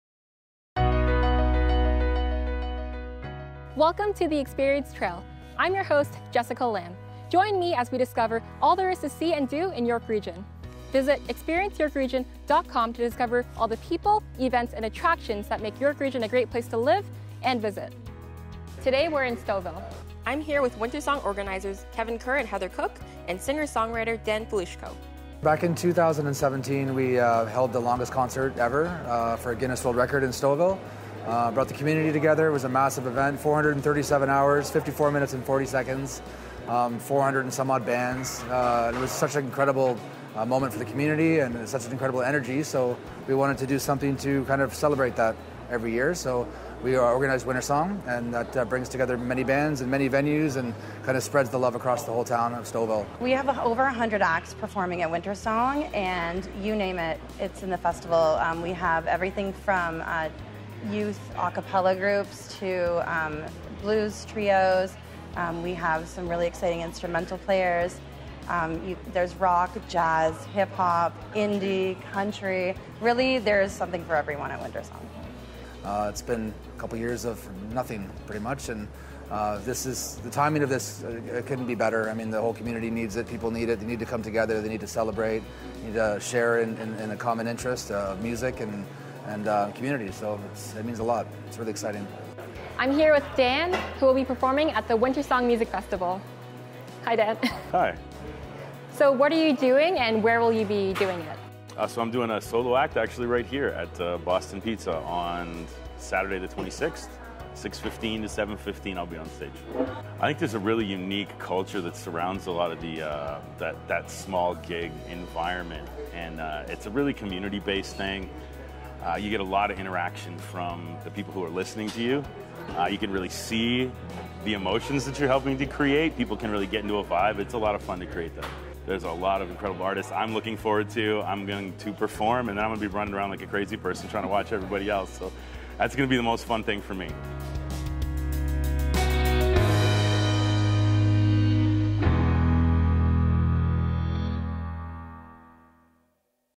Experience Trail - Stouffville Winter Song 2022
Come join us along the Experience Trail!

The Experience Trail will re-connect York Region residents – and visitors – to the unique and interesting places that dot the region and bring our local culture to life.

We’ve crisscrossed York Region, capturing the places, events and people who make York Region such an amazing place to live and visit.

The Warm Hearts Collective and the team behind the Guinness World Record for “The Longest Concert by Multiple Artists” have joined forces once again for Wintersong 2022, an exciting community festival for families and music lovers of all ages.

On March 25 and March 26, Stouffville, ON will be woken from hibernation by a diverse lineup of indie musicians, installations and engaging interactive experiences. This community event will attract attendees from across York Region and beyond for two days of music, shopping and dining.